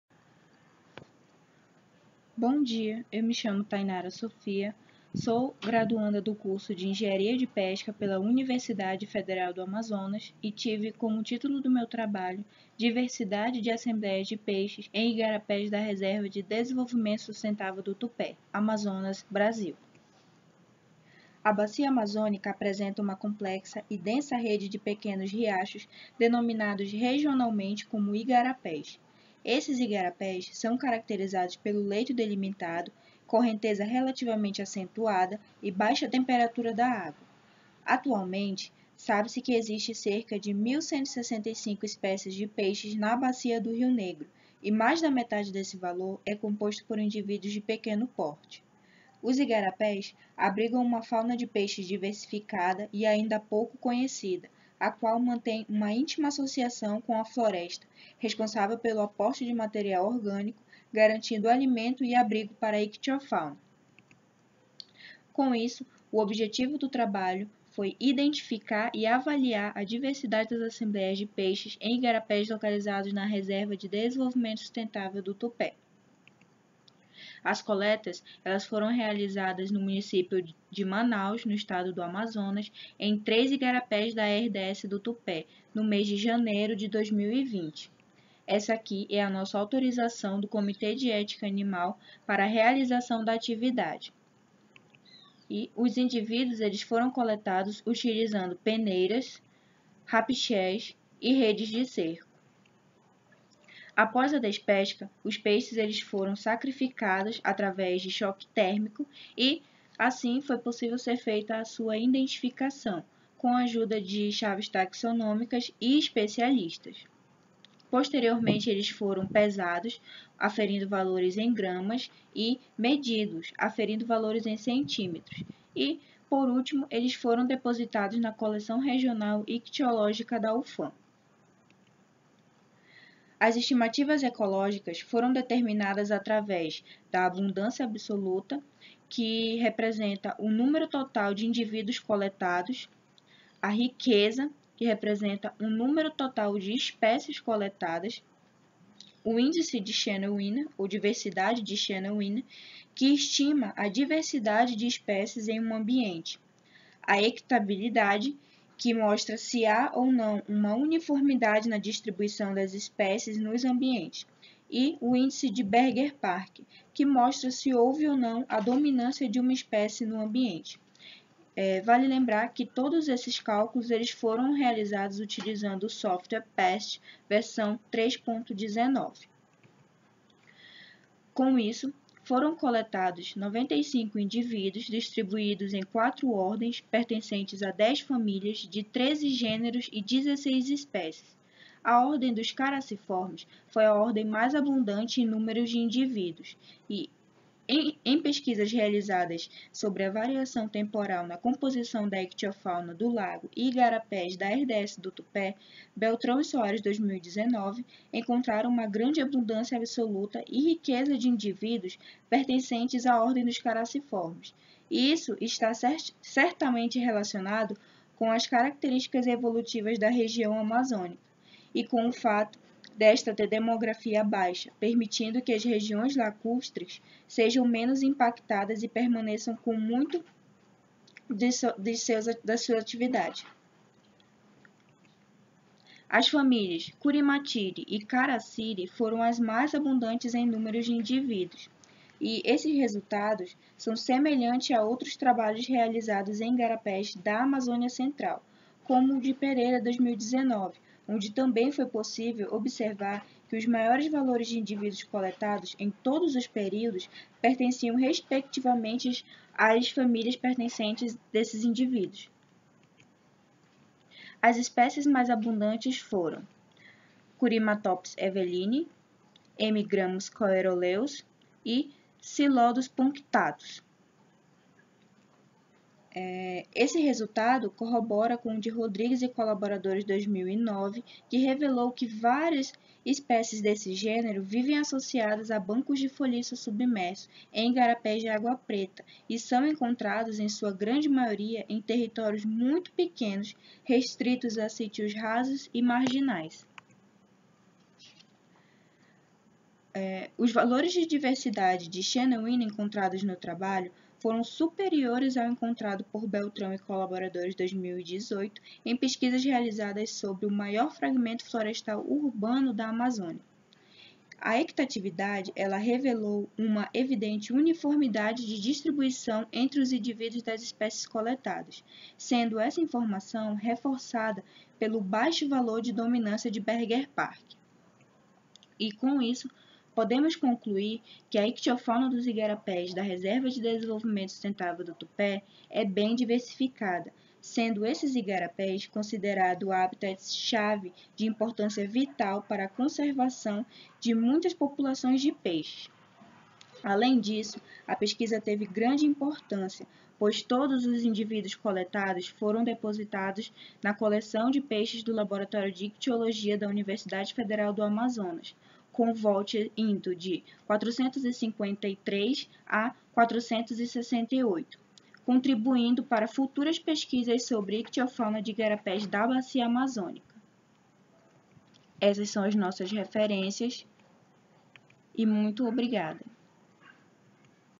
S66 - Apresentação Thaynara Sofia Gomes Vieira
DIVERSIDADE DE ASSEMBLEIAS DE PEIXES EM IGARAPÉS DA RESERVA DE DESEMVOLVIMENTO SUSTENTÁVEL DO TUPÉ, AMAZONAS – BRASIL